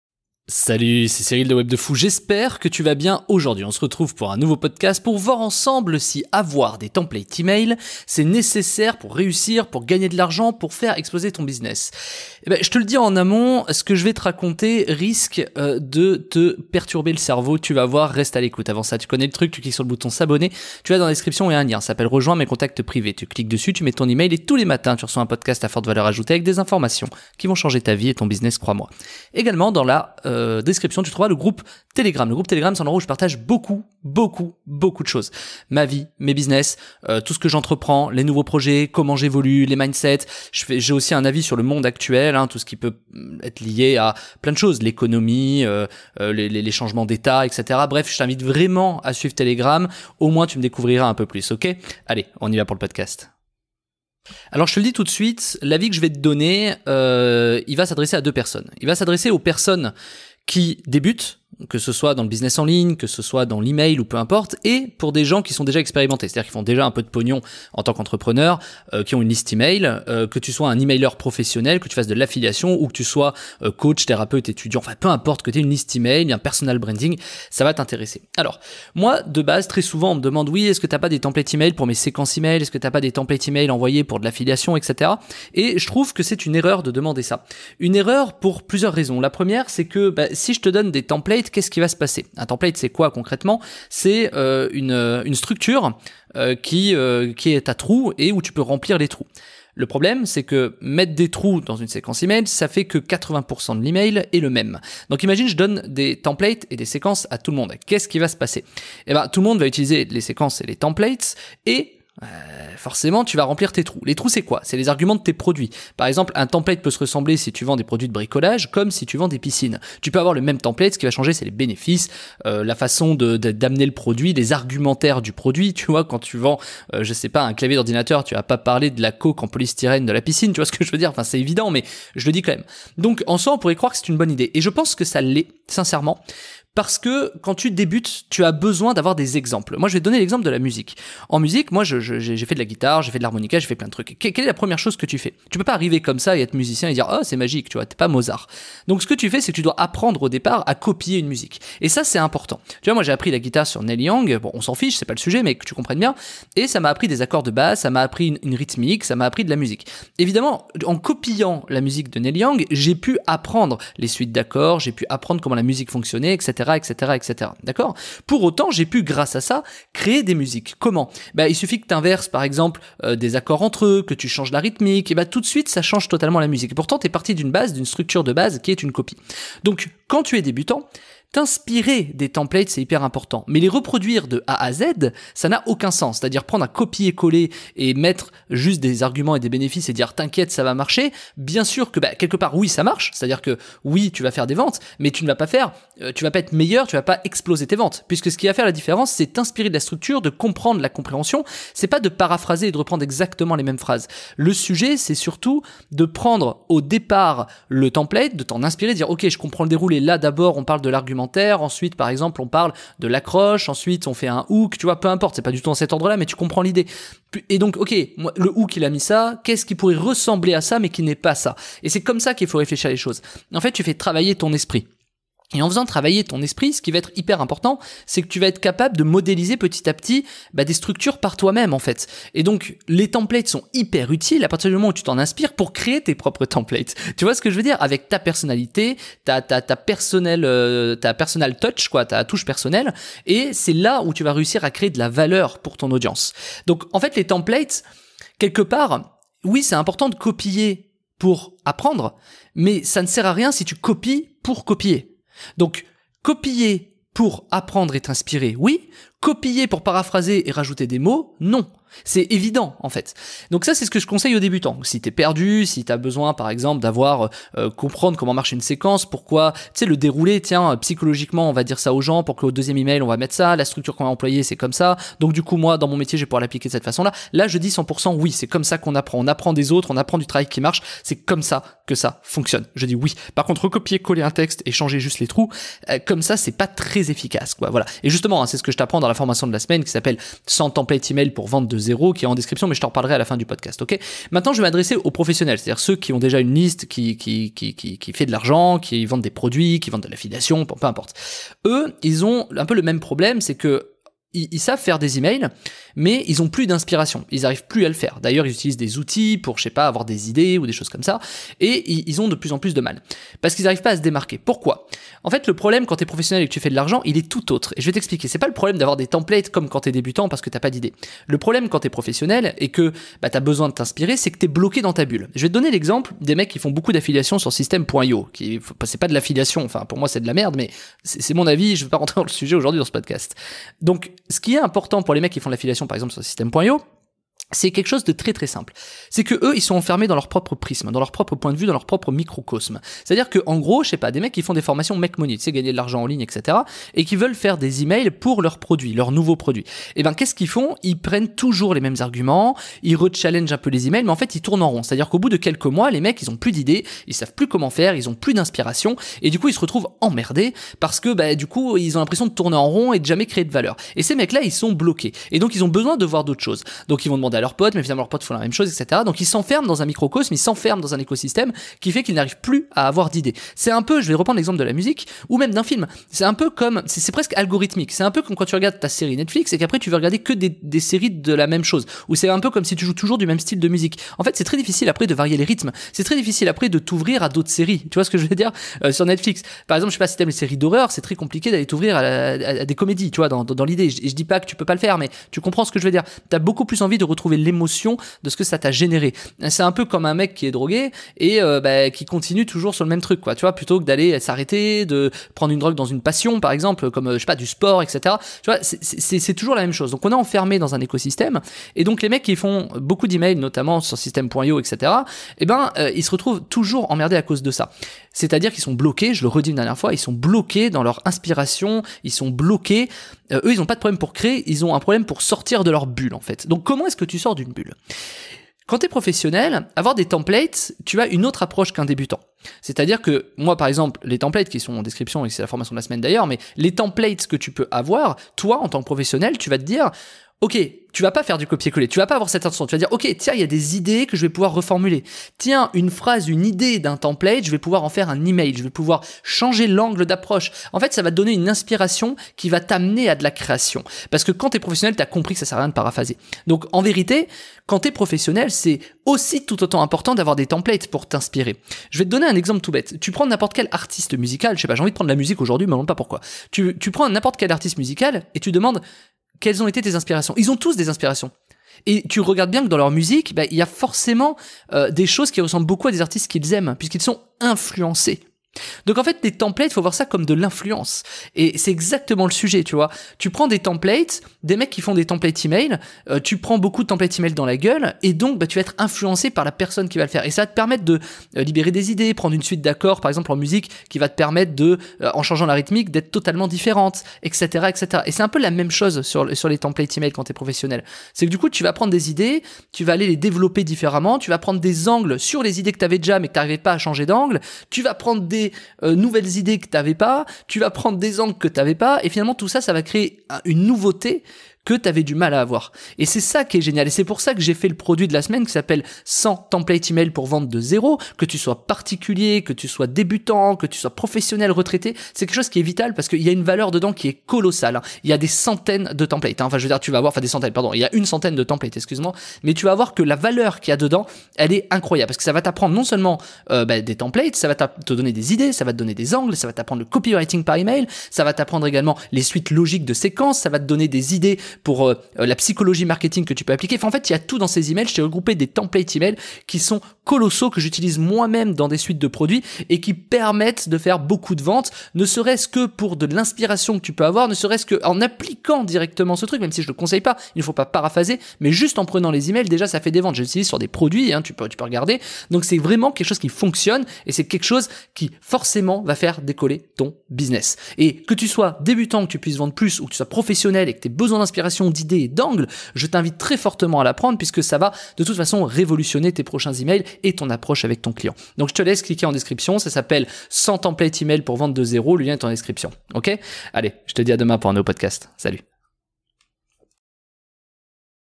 972 Web2fou fr Les templates emails sont necessaires pour reussir podcast youtube
Salut c'est Cyril de web2fou.fr

Quand on commence on a besoin d'exemples

✅ Fini le manque d'inspiration et d'idées
✅ Ce que je conseille aux débutants & pros en emails
✅ Exemple et parralèle entre l'email et la musique

Je te donne mon avis et ce que je conseille en email peu importe ton niveau